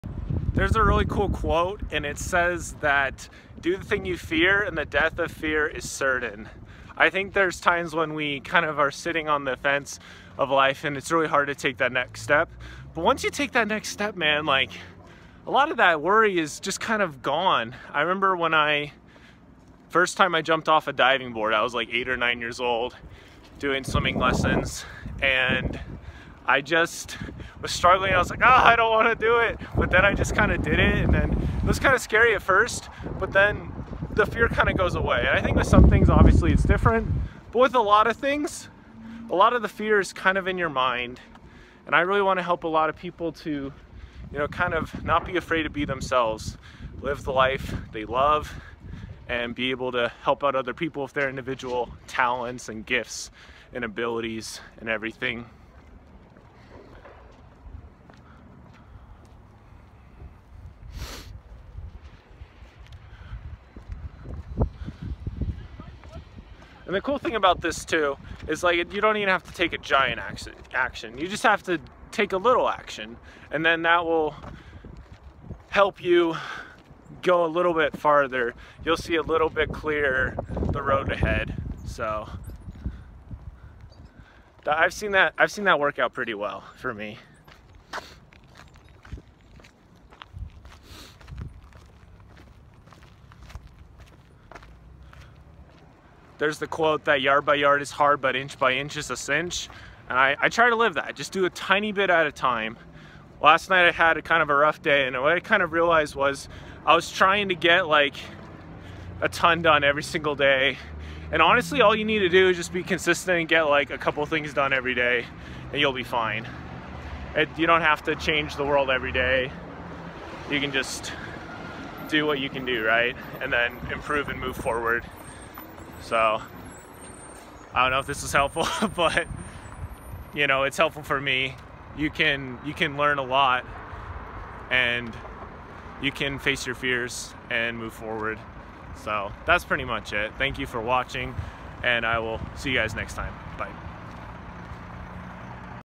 Do the Thing You Fear (and the Death of Fear is Certain)
I recorded this 3 years ago back in college and never uploaded it… and today I rediscovered it.

This is 100% the message I needed to hear today. I hope it is helpful for you too 💪🏻

Check out my first book, “God is Trying to Talk to Me”